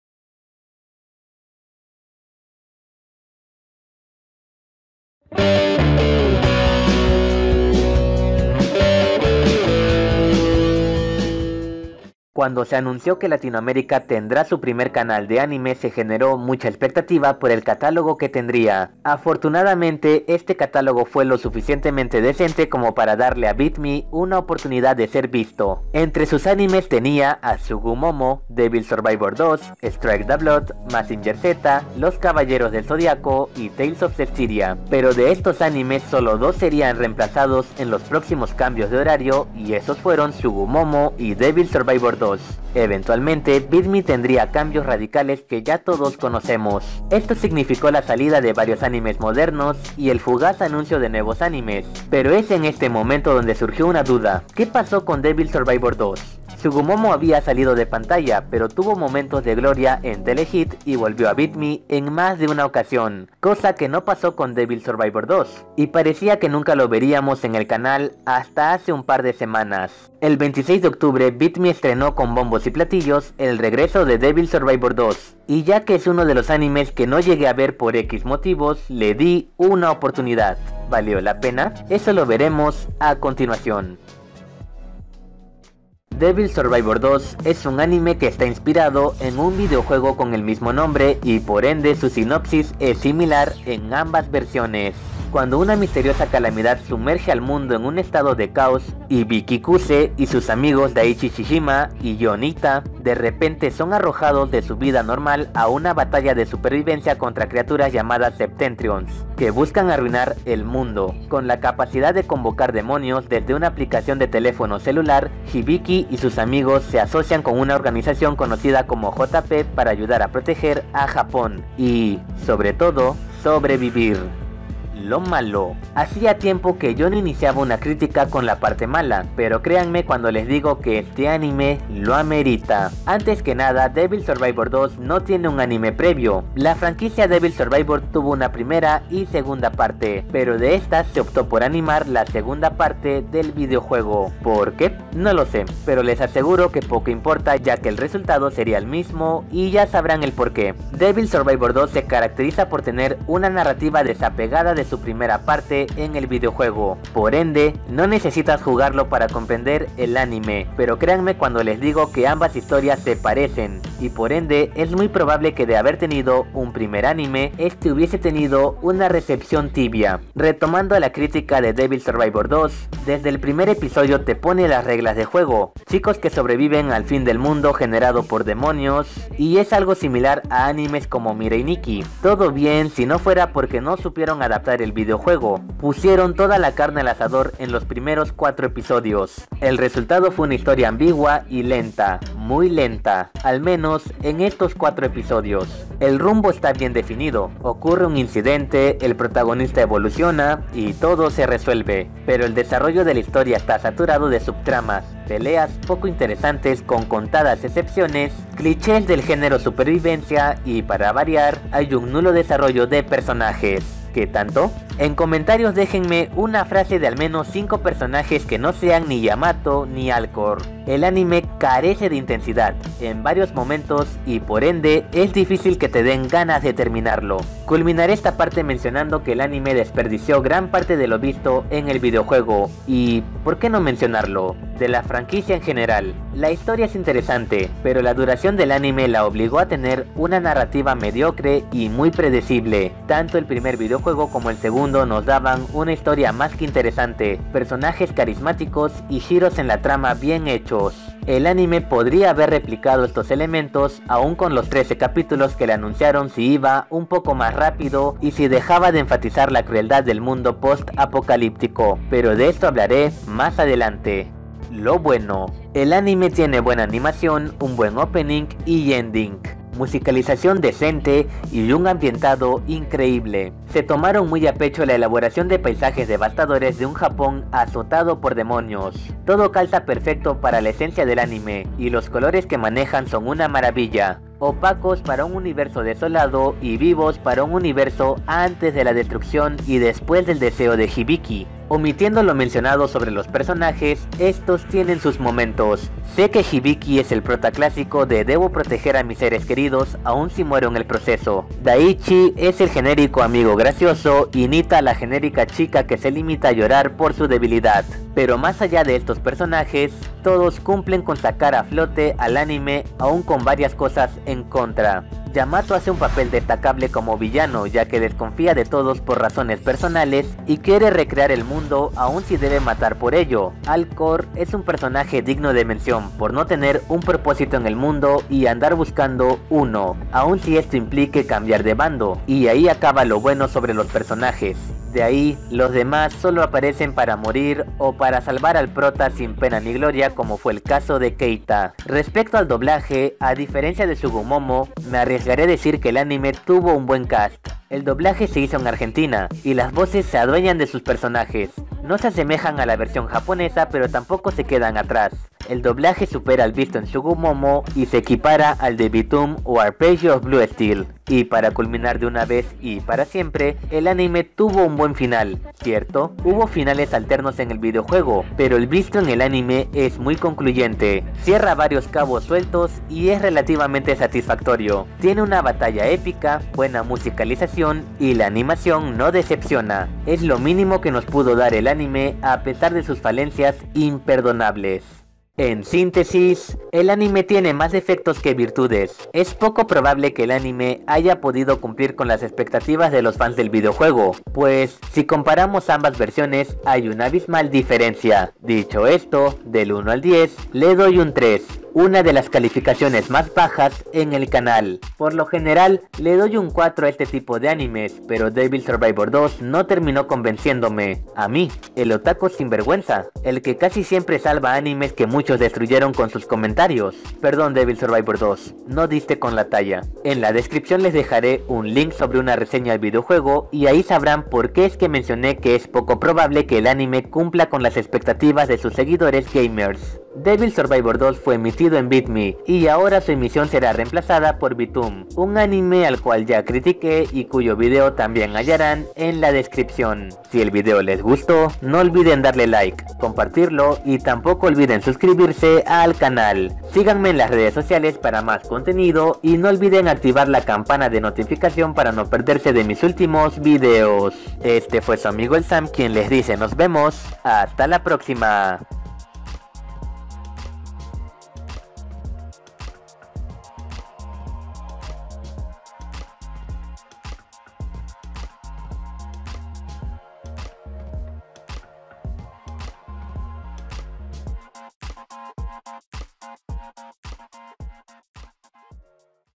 DEVIL SURVIVOR 2: No le hace honor al videojuego
BitMe ha finalizado la emisión del anime Devil Survivor 2, también conocido como Devil Survivor 2: The Animation, y por consiguiente me vi en la obligación de brindarles este vídeo sobre los aspectos destacables de la serie. Sin embargo, eso es algo que Devil Survivor 2 solo cumplió en su videojuego dado a que la realidad en su anime fue otra.

2:04 Lo malo
4:24 Lo bueno

Btooom!
Sigan a Avii por Instagram, ella es la mente maestra detrás del fantástico perfil del canal.
Devil Survivor 2 (también conocido como Shin Megami Tensei: Devil Survivor 2) es un videojuego desarrollado por Atlus y cuya adaptación anime (Devil Survivor 2: The Animation) corrió a cargo del estudio Bridge. Fue emitido por el canal BitMe. Si quieres ver más animes mediante este canal entonces no dudes en revisar su programación.